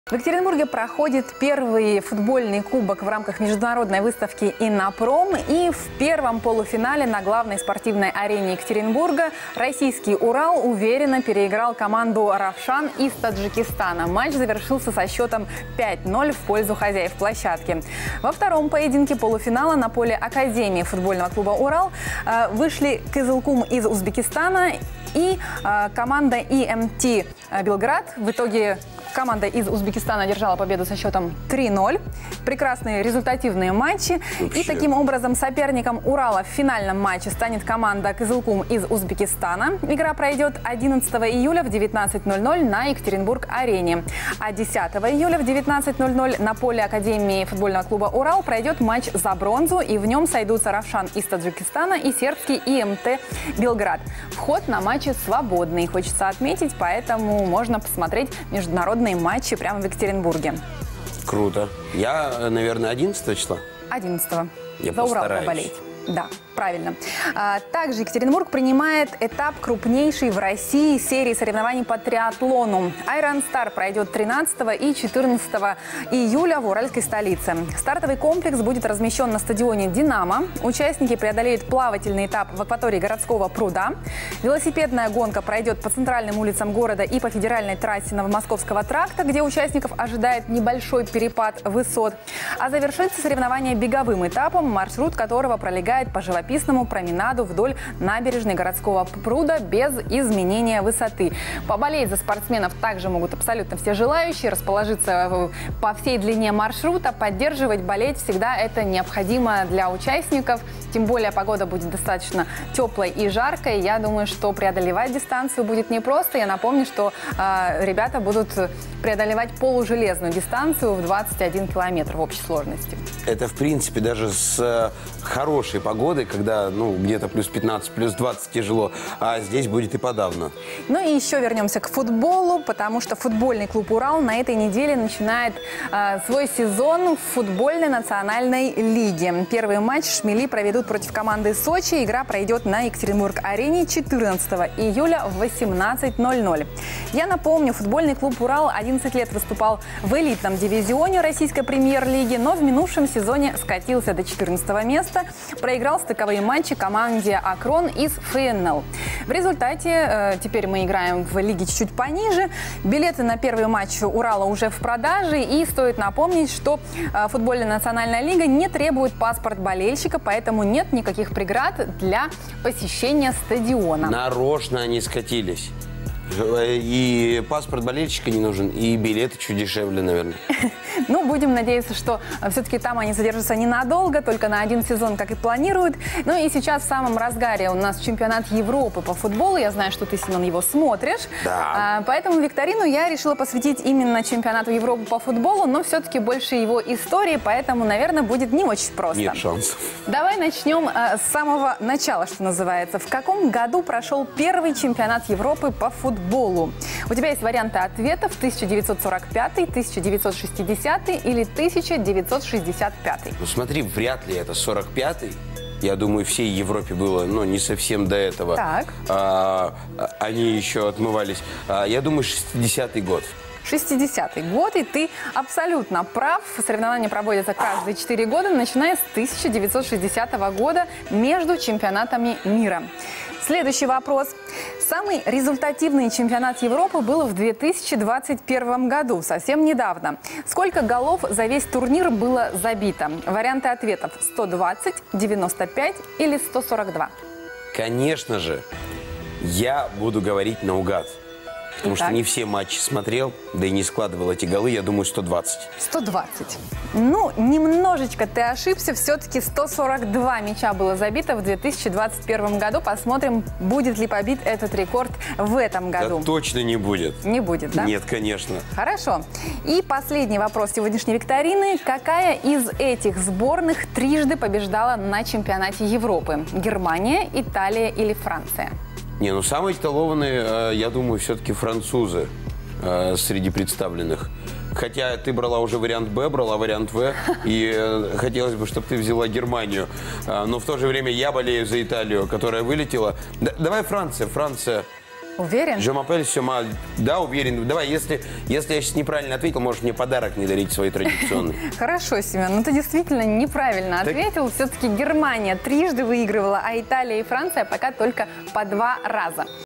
БЕГ С МЯЧОМ И БЕЗ: Чем удивил мир спорта на этот раз? 8.07.2024
Лето в Екатеринбурге насыщено спортивными событиями: футбольный Кубок ИННОПРОМа, соревнования по триатлону IRONSTAR, первые матчи ФК «Урал» в Футбольной национальной лиге... ⚽🏆 Все актуальные новости летней спорт-программы - в рубрике «Спорт-подход»! И, конечно, в конце вас ждет традиционная викторина от Татьяны Кузнецовой.